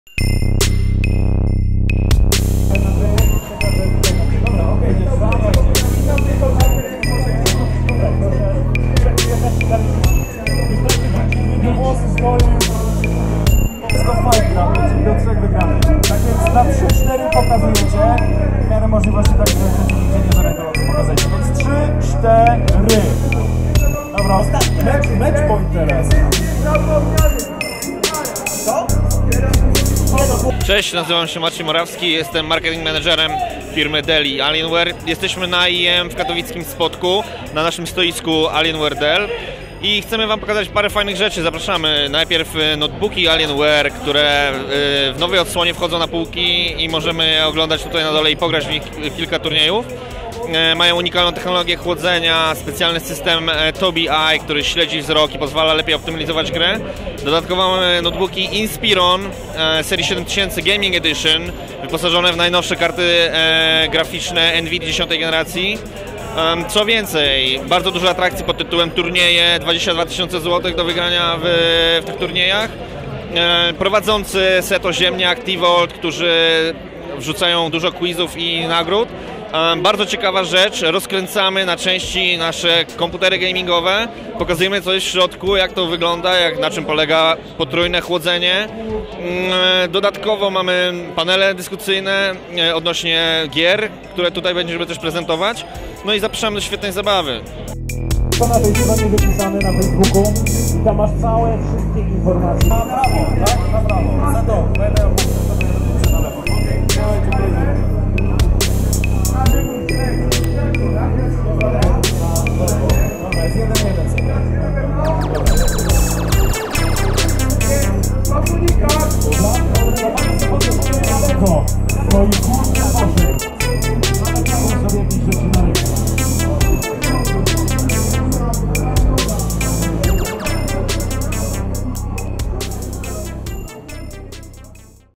Dell prezentuje dwa nowe Inspirony
Wśród wystawców na Intel Extrem Masters nie mogło zabraknąć takiej marki jak Dell Alienwere. Wśród prezentowanych różnych sprzętów z logo obcego znalazły się też dwie prawdziwe perełki z logo Dell. To notebooki z serii Inspiron, jednak z zacięciem gamingowym. Dokładnie mowa o Dell Inspiron z serii 7000 o rozmiarach 15 i 17 cali.